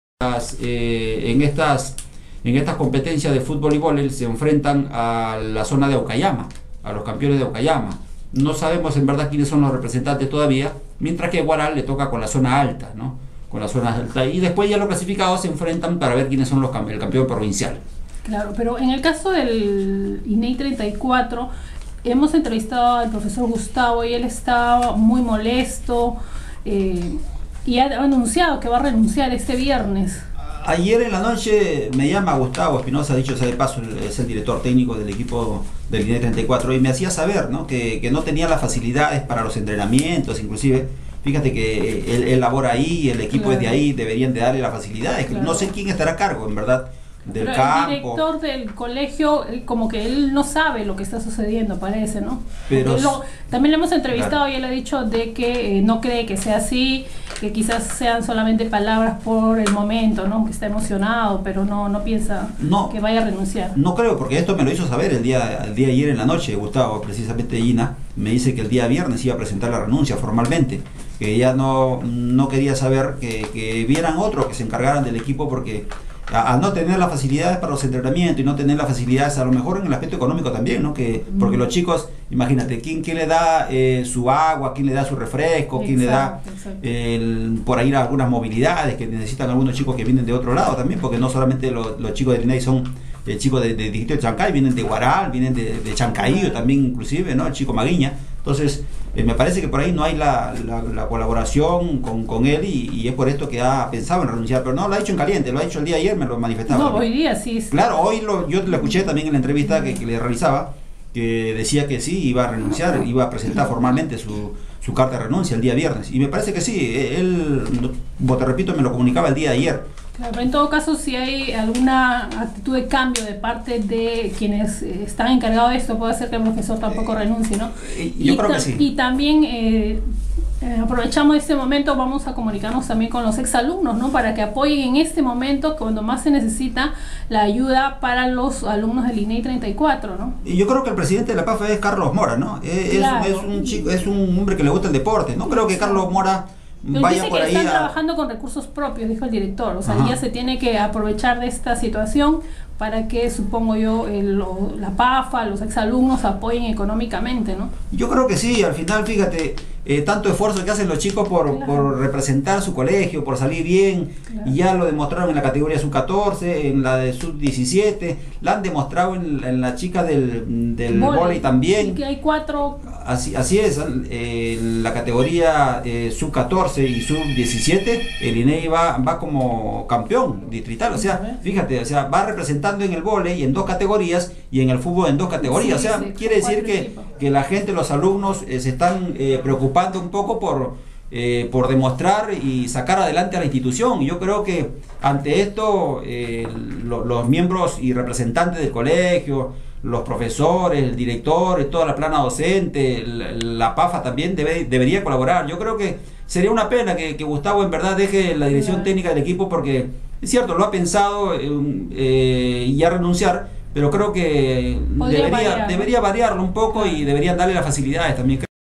Chancay: Comentario participación del INEI en Juegos Escolares
11 julio 2012.- Prof. Garri Narvarte comenta participación del colegio INEI 34 en los Juegos Deportivos Escolares en Chancay. Vídeo: Georgina Carlos